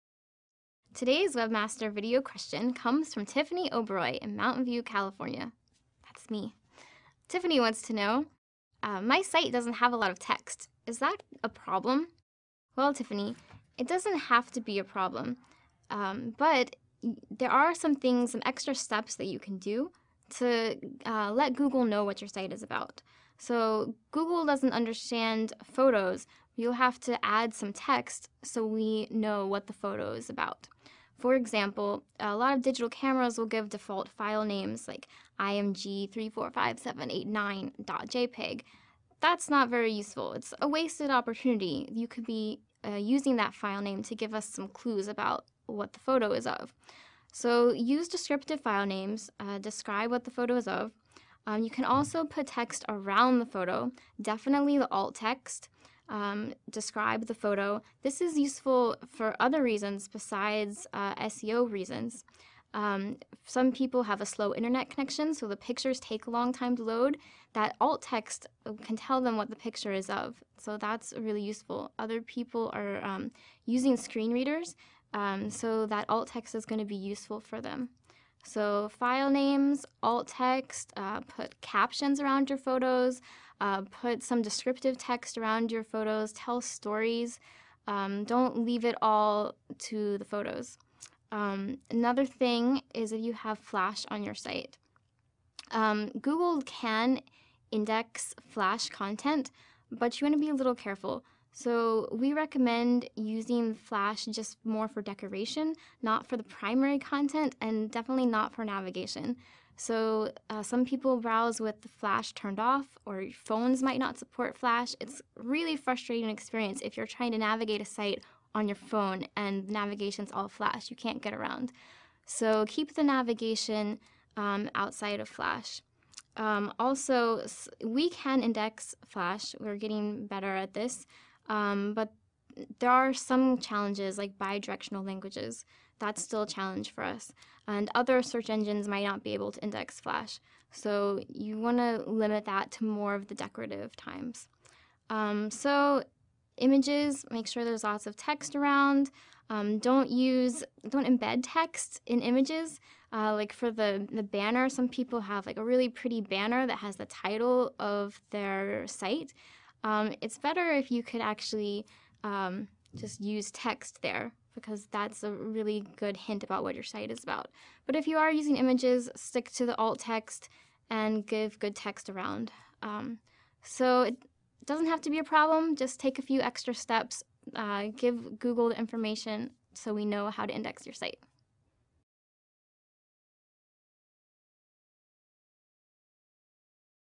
Comment optimiser le référencement d'un site avec peu de texte ?
Dans une vidéo officielle, Google fournit quelques conseils pour les sites ayant très peu de contenu texte, par exemple ceux avec principalement des photos ou du Flash.